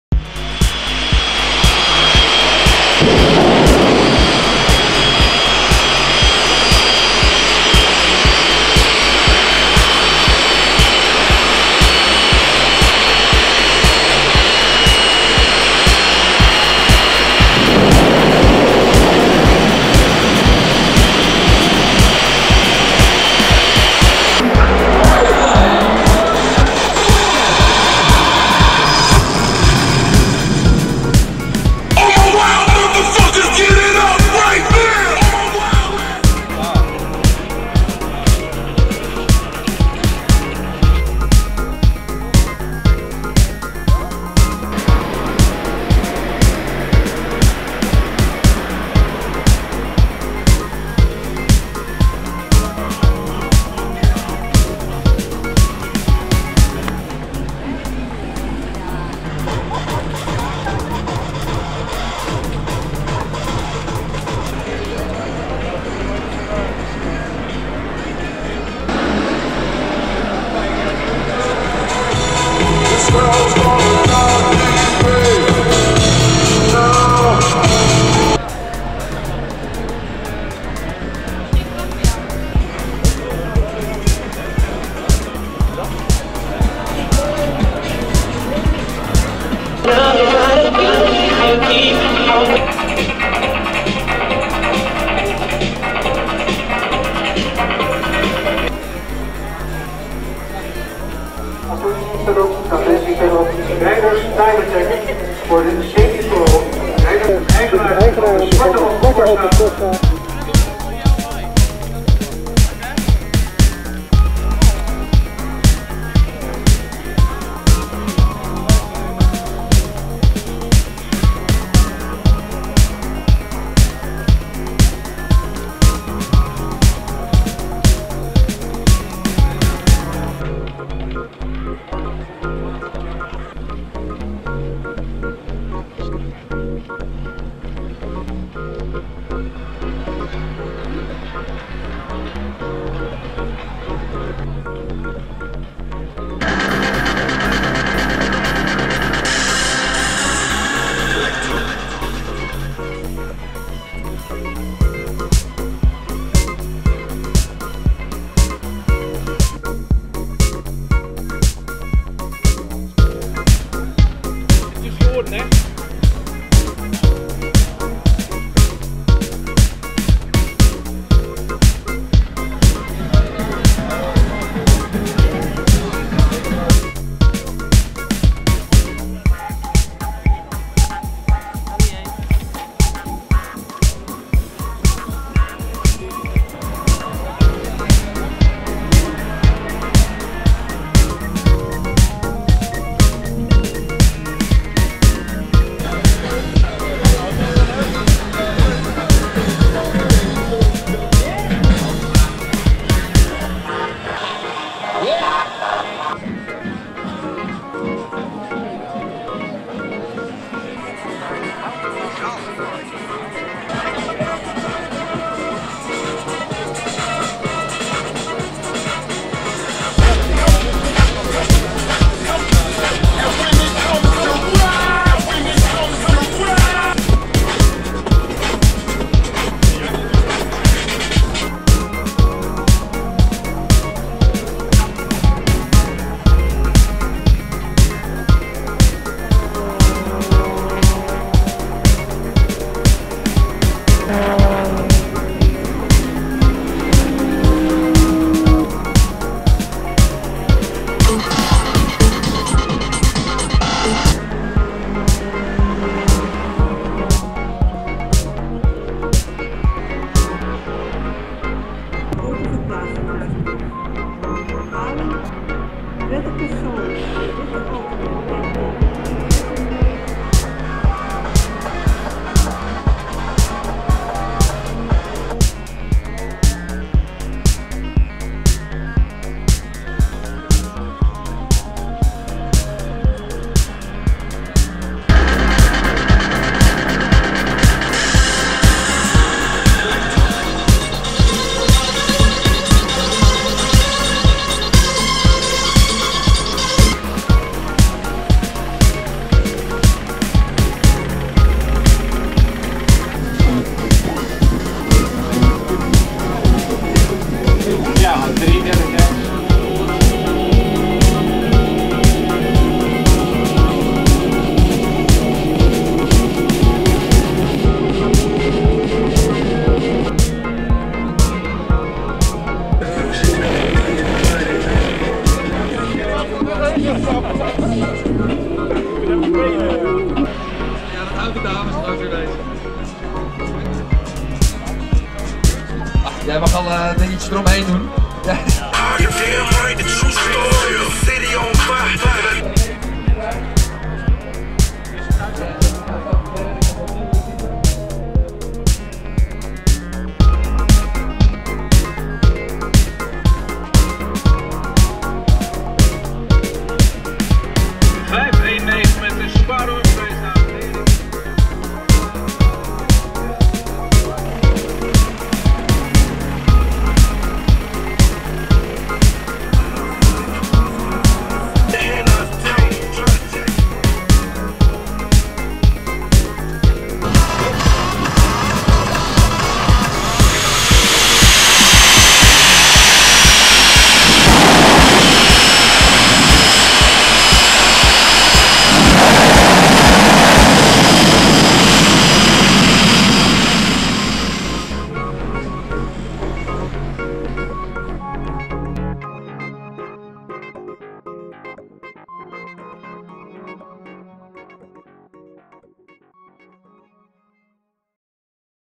Automaxx streetpower 2012 Assen
Impressie van de Automaxx Streetpower in Assen op 1 april 2012, beelden van alle activiteiten en actie